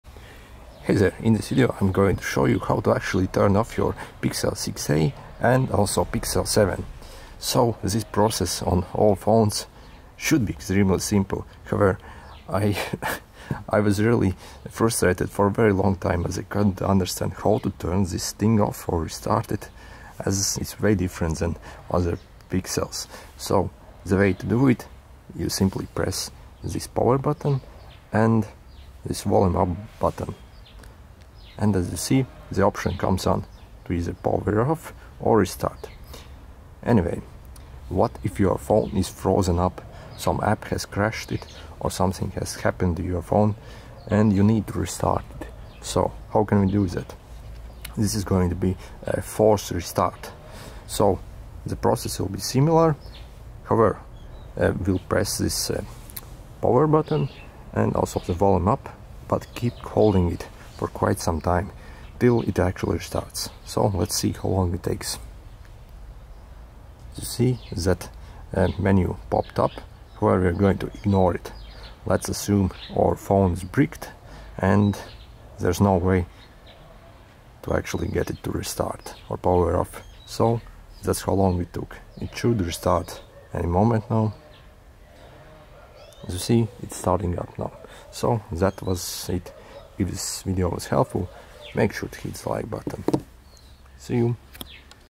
Google Pixel 6a / Pixel 7 - How to Turn OFF ,Restart & Force Restart
How to turn off your google pixel 6a and pixel 7 phone, also how to restart it or force restart your google pixel 7 or pixel 6a